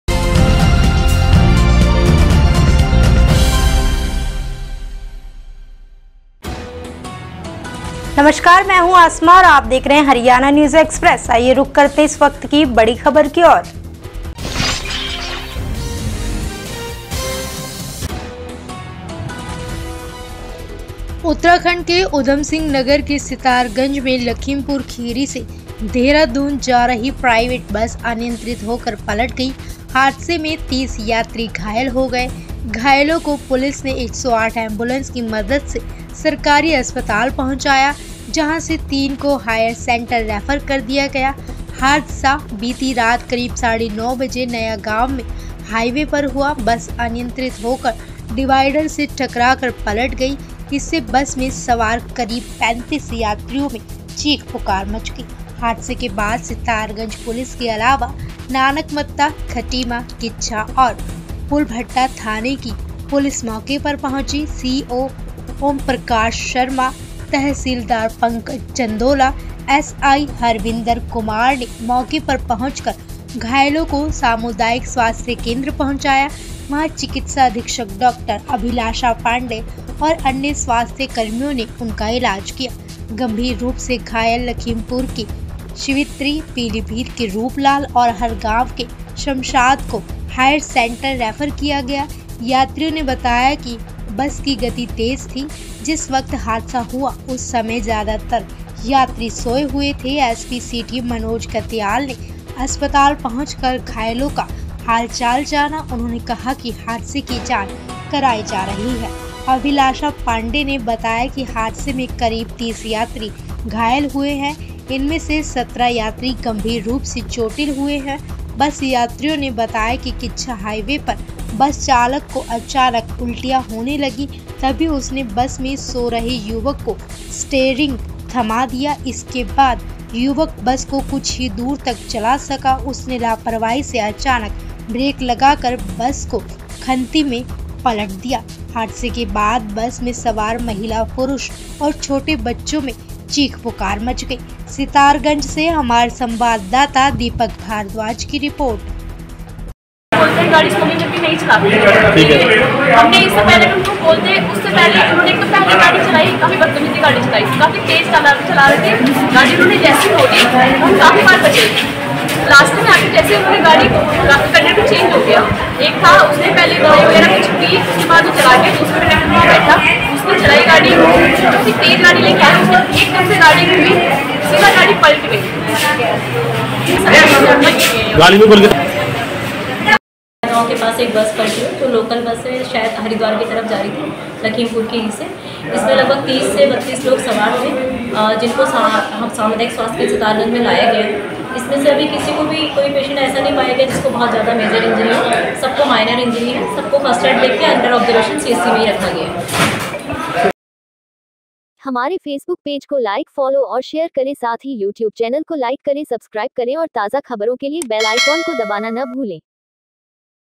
उत्तराखंड : बेकाबू बस पलटने से घायल हुए 30 यात्री
उत्तराखंड के उधम सिंह नगर के सितारगंज में लखीमपुर खीरी से देहरादून जा रही प्राइवेट बस अनियंत्रित होकर पलट गई। हादसे में 30 यात्री घायल हो गए। घायलों को पुलिस ने 108 एंबुलेंस की मदद से सरकारी अस्पताल पहुंचाया। जहां से तीन को हायर सेंटर रेफर कर दिया गया। हादसा बीती रात करीब साढ़े 9 बजे नयागांव में हाईवे पर हुआ। बस अनियंत्रित होकर डिवाइडर से टकराकर पलट गई। इससे बस में सवार करीब 35 यात्रियों में चीख पुकार मच गई। हादसे के बाद सितारगंज पुलिस के अलावा नानकमत्ता, खटीमा, किच्छा और पुलभट्टा थाने की पुलिस मौके पर पहुंची। सीओ ओमप्रकाश शर्मा, तहसीलदार पंकज चंदोला, एसएसआई हरविंद्र कुमार ने मौके पर पहुंचकर घायलों को सामुदायिक स्वास्थ्य केंद्र पहुंचाया। वहां चिकित्साधीक्षक डॉ. अभिलाषा पांडेय और अन्य स्वास्थ्य कर्मियों ने उनका इलाज किया। गंभीर रूप से घायल लखीमपुर की शिवित्री, पीलीभीत के रूपलाल और हरगांव के शमशाद को हायर सेंटर रेफर किया गया है। यात्रियों ने बताया कि बस की गति तेज थी। जिस वक्त हादसा हुआ उस समय ज्यादातर यात्री सोए हुए थे। एसपी सिटी मनोज कत्याल ने अस्पताल पहुंचकर घायलों का हालचाल जाना। उन्होंने कहा कि हादसे की जांच कराई जा रही है। अभिलाषा पांडे ने बताया कि हादसे में करीब 30 यात्री घायल हुए हैं। इनमें से 17 यात्री गंभीर रूप से चोटिल हुए हैं। बस यात्रियों ने बताया कि किच्छा हाईवे पर बस चालक को अचानक उल्टियां होने लगी तभी उसने बस में सो रहे युवक को स्टेरिंग थमा दिया। इसके बाद युवक बस को कुछ ही दूरी तक चला सका। उसने लापरवाही से अचानक ब्रेक लगाकर बस को खंती में पलट दिया। हादसे के बाद बस में सवार महिला पुरुष और छोटे बच्चों में चीख पुकार मच गई।